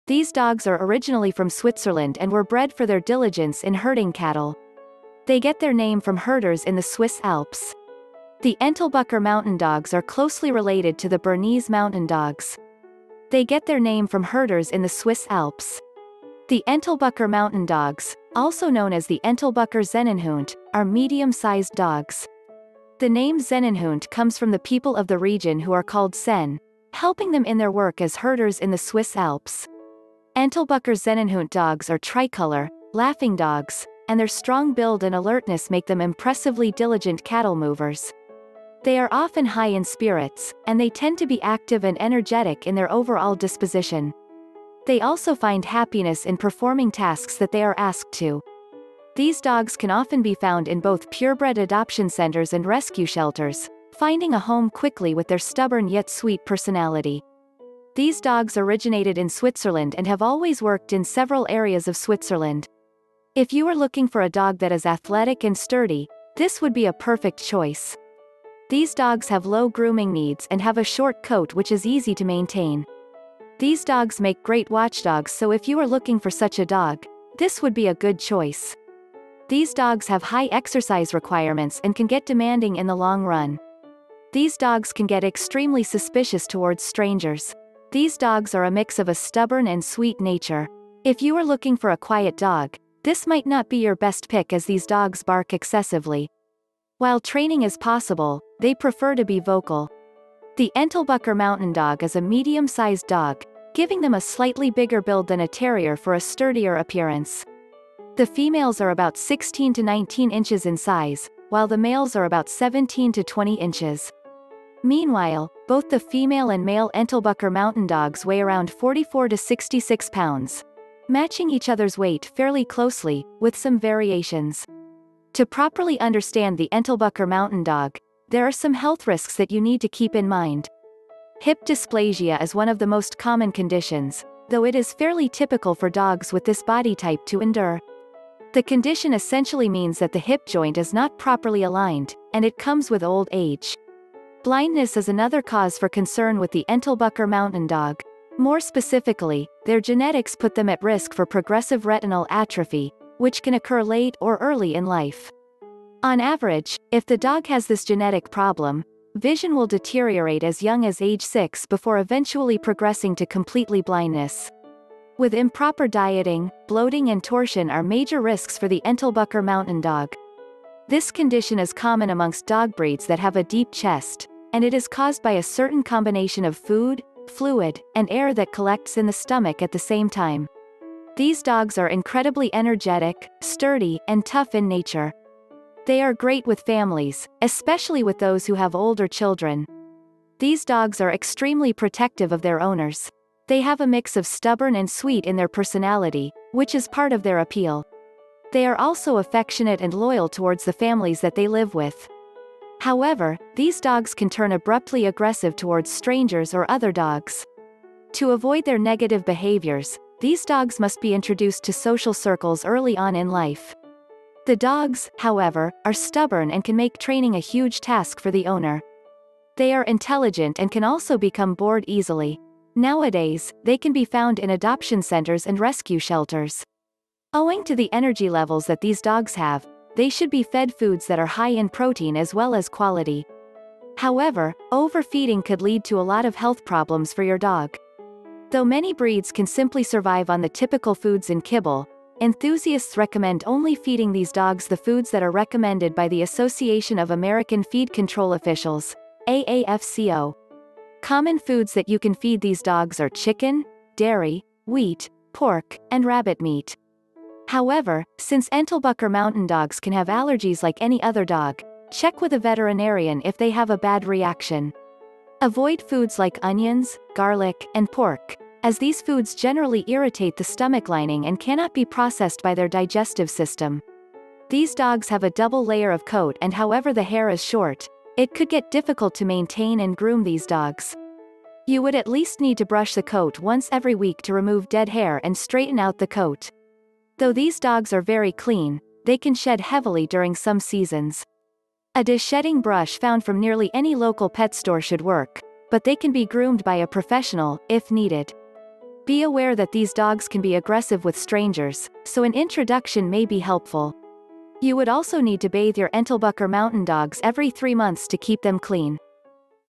Entlebucher Mountain Dog are originally from Switzerland and were bred for their diligence in herdin
These dogs Entlebucher Mountain Dog are originally from Switzerland and were bred for their diligence in herding cattle. They get their name from herders in the Swiss Alps. The Entlebucher mountain dogs are closely related to the Bernese mountain dogs. They get their name from herders in the Swiss Alps. The Entlebucher mountain dogs, also known as the Entlebucher Sennenhund, are medium-sized dogs. The name Sennenhund comes from the people of the region who are called Senn, helping them in their work as herders in the Swiss Alps.